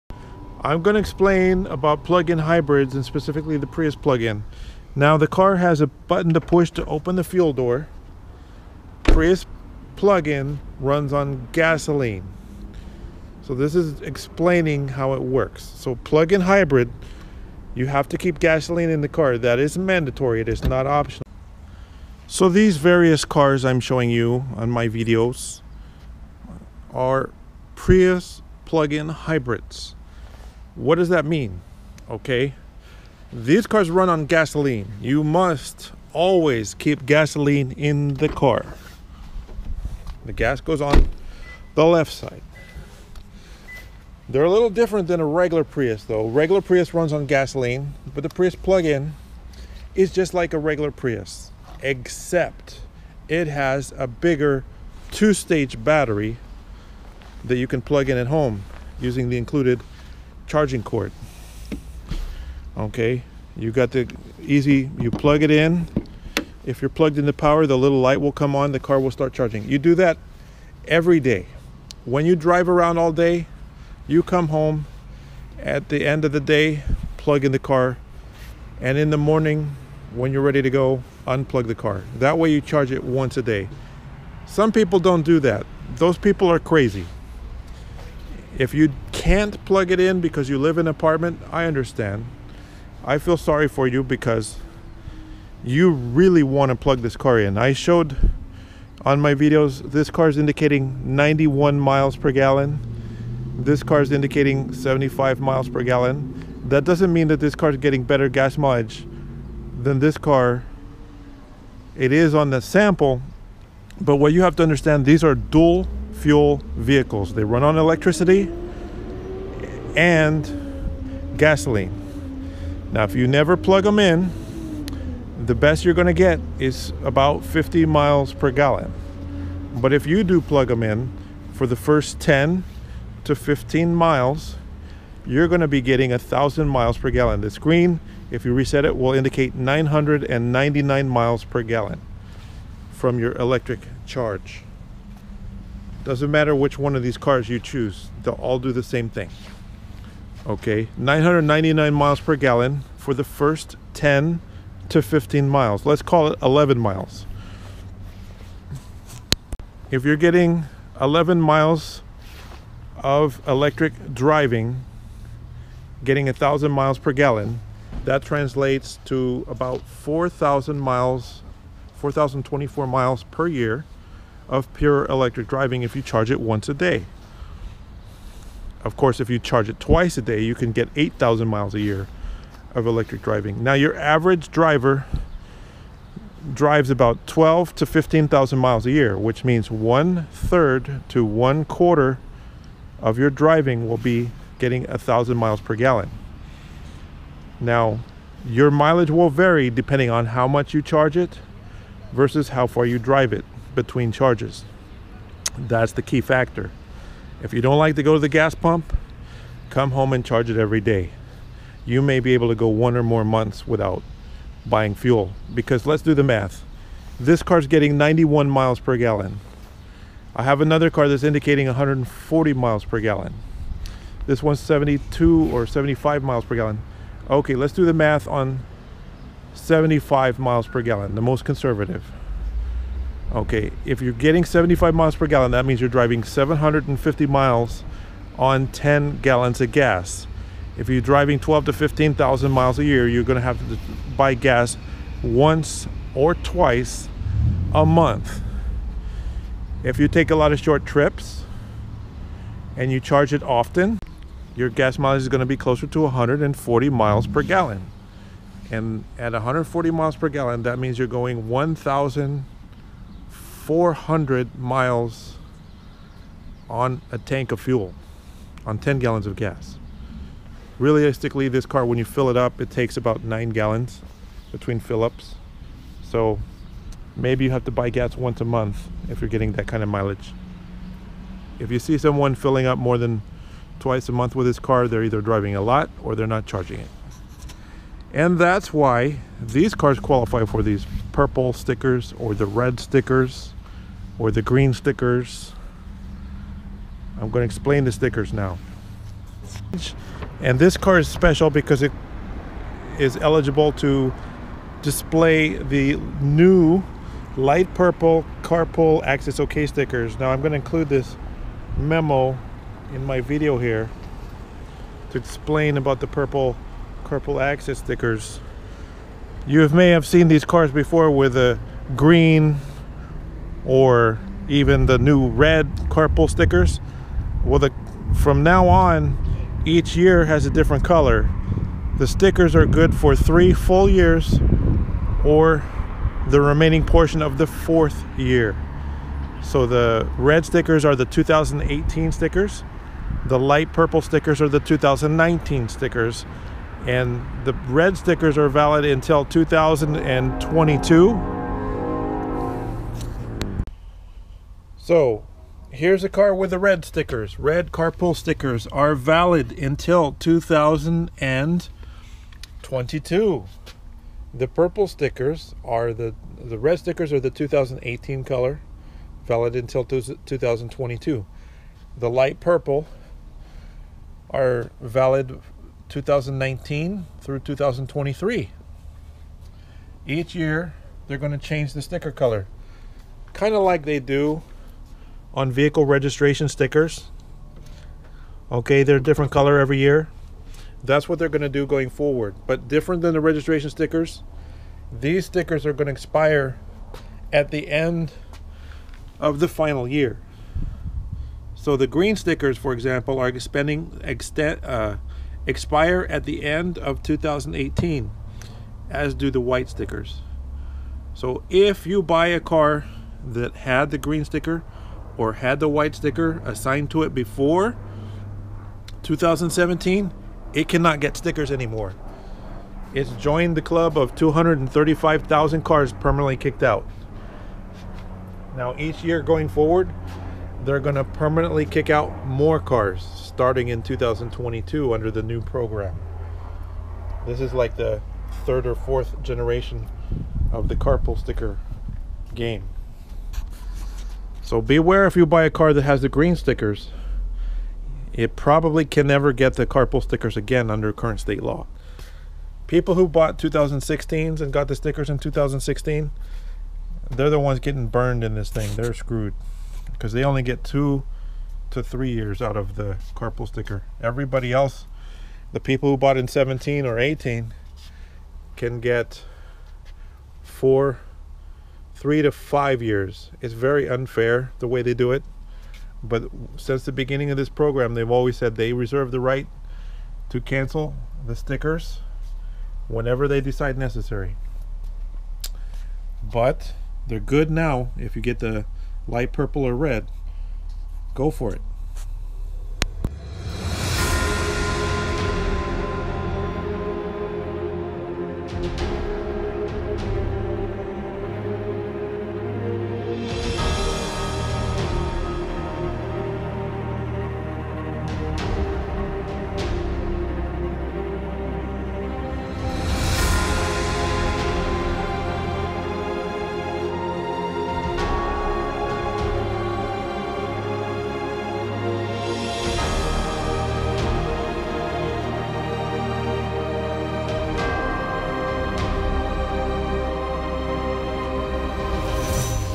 HOV Stickers And Plug-in Hybrids Explained
How to Understand about Carpool Stickers. The Basics, Purple, Green, Red, White. and what is the difference between a Plug-in Hybrid and a regular Hybrid?